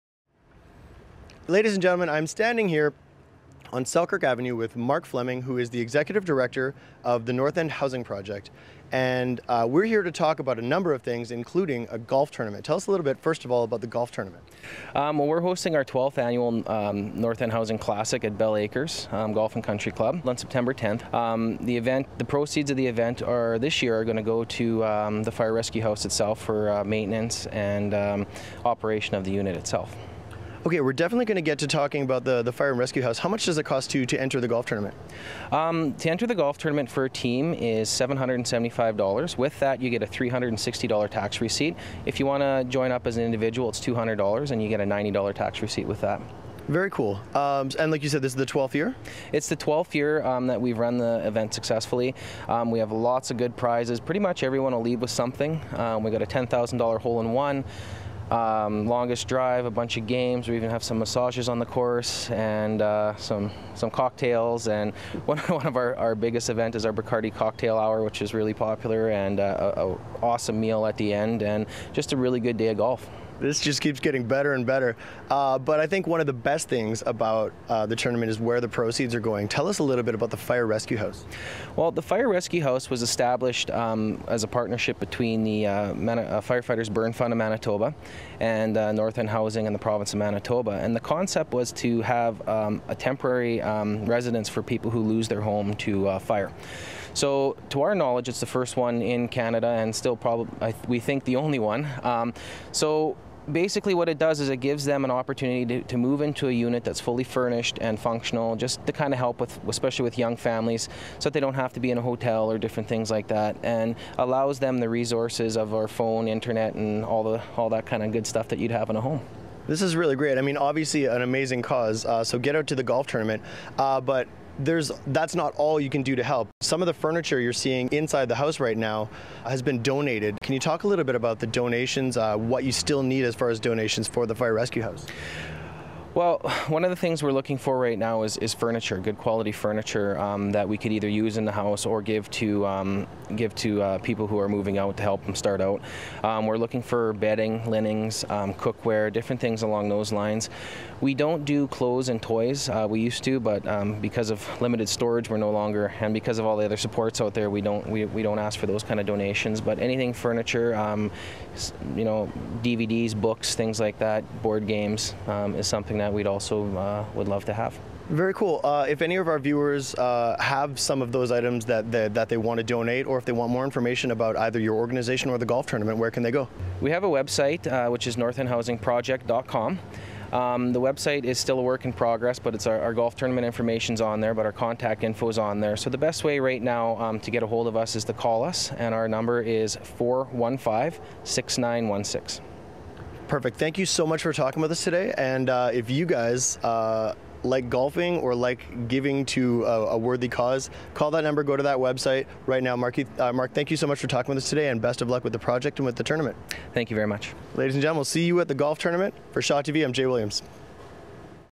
North End Housing Classic 2012
Shaw TV's J.Williams speaks to Mark Fleming about the 2012 NOrth End Housing Classic Golf Tournament and about Fire Rescue House in Winnipeg